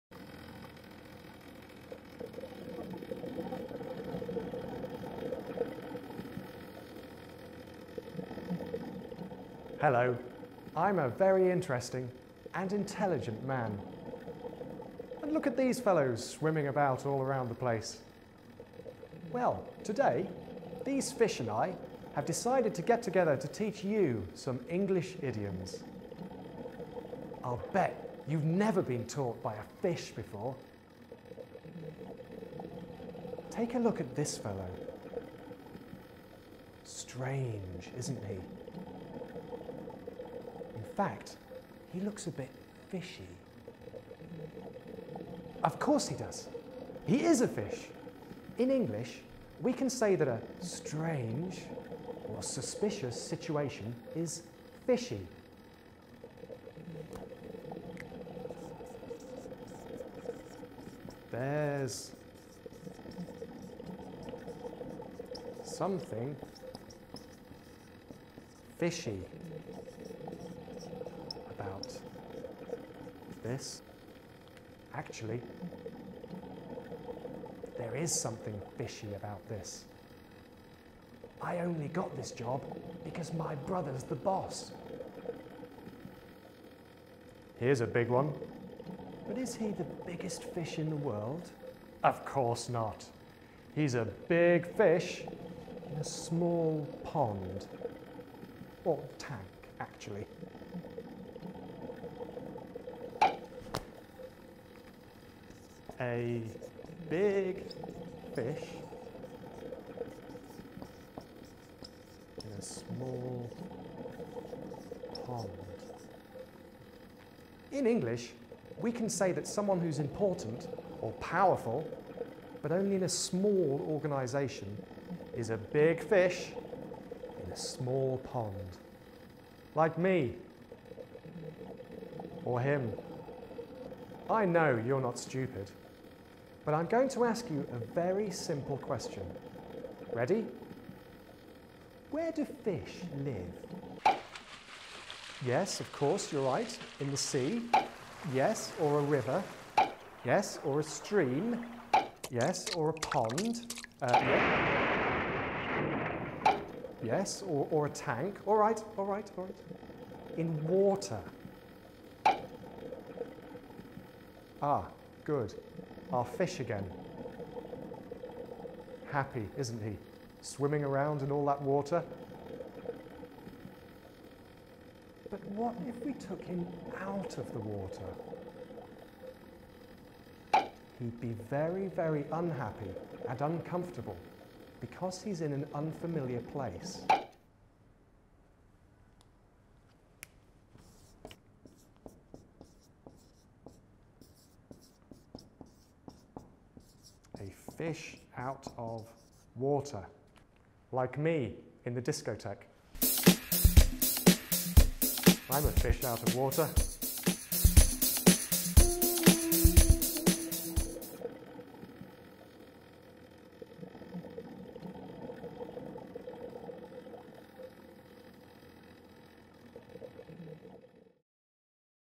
Idioms (Fish)\ Английские идиомы
1. There's something fishy about this
2. A big fish in a small pond
3. A fish out of water